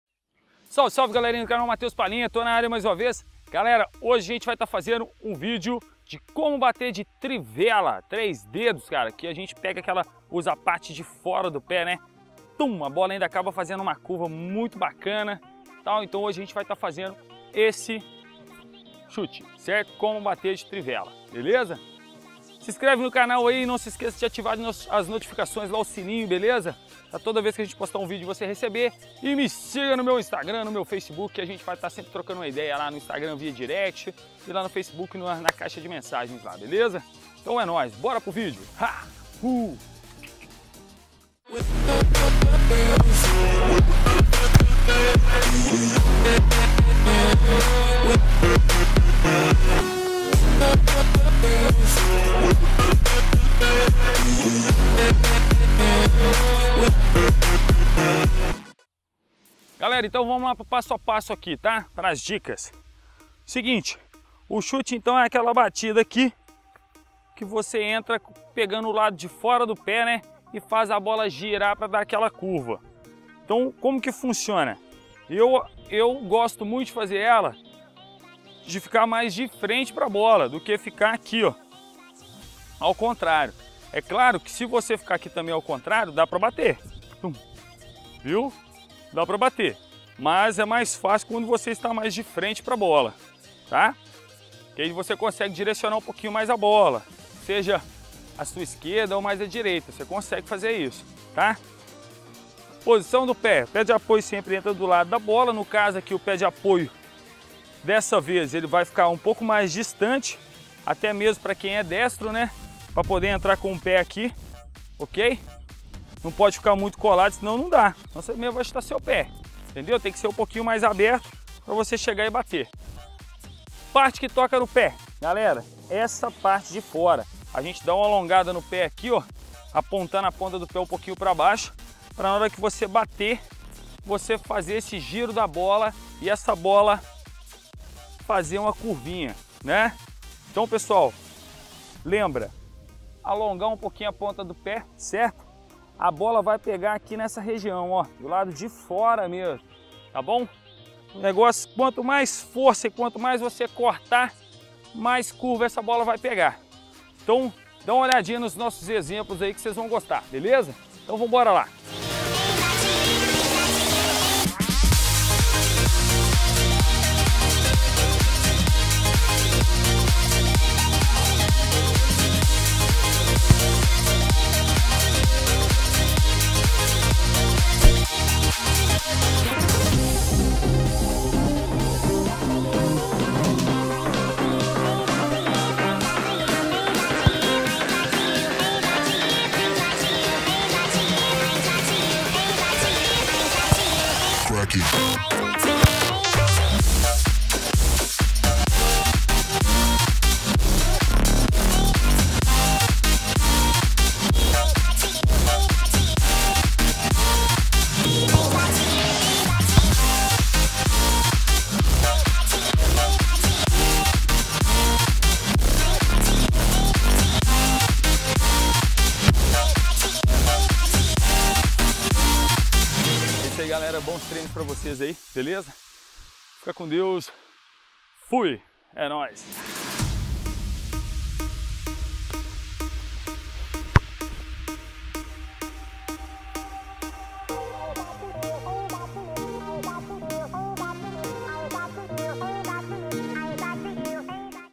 APRENDA A CHUTAR DE TRIVELA
Salve, galera! Vídeo de hoje é como CHUTAR DE TRIVELA, dicas importantes. Assista tudo e treine bastante para vc arrebentar!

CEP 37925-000
Piumhi-MG